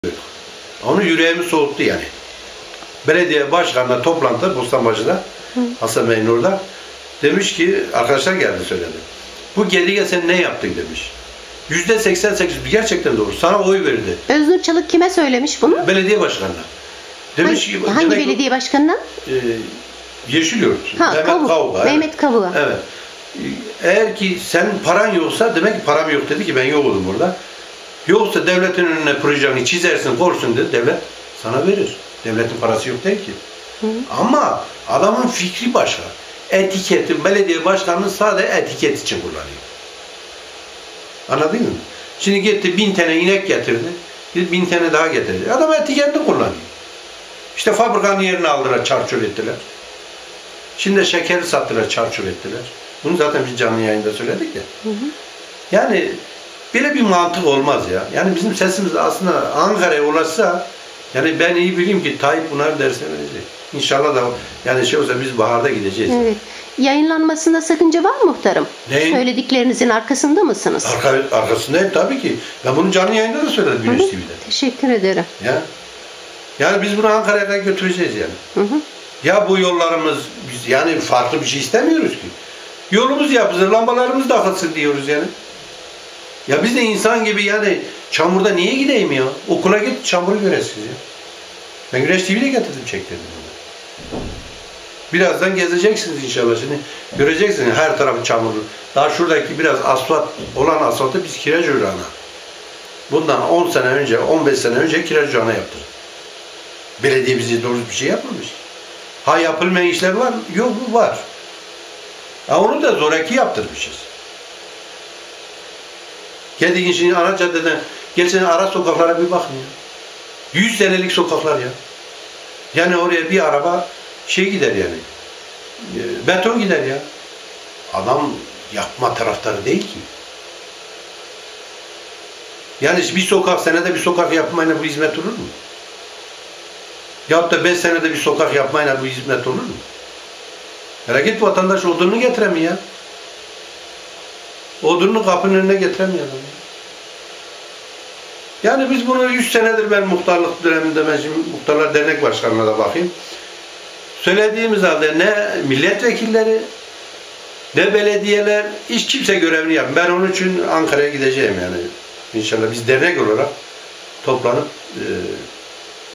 AKP'liden AKP'liye ağır suçlamalar-Malatya
2 Mart 2012 Malatya Yeşilyurt Gedik Mahallesi Muhtarı Abuzer Taylan'ın evinde
AKP'liden AKP'liye ağır suçlamalar: Gedik Mahallesi Muhtarı Abuzer Taylan da Yeşilyurt Belediye Başkanı Mehmet Kavuk da AKP'li...